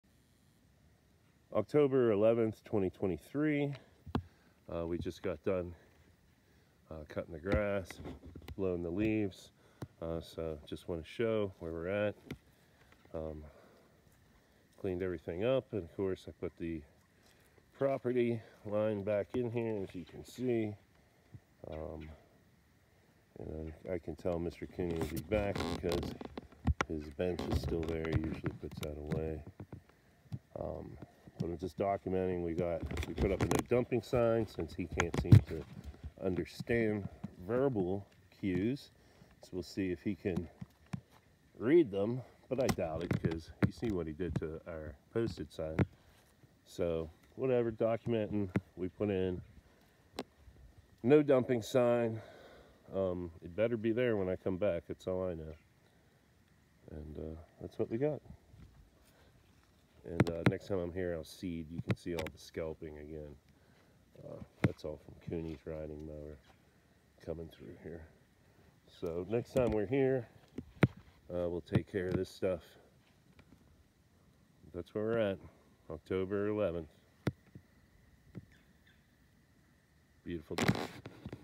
10/11/2023 Maintenance Aftermath NO DUMPING
Put up a NO DUMPING SIGN. documenting maintenance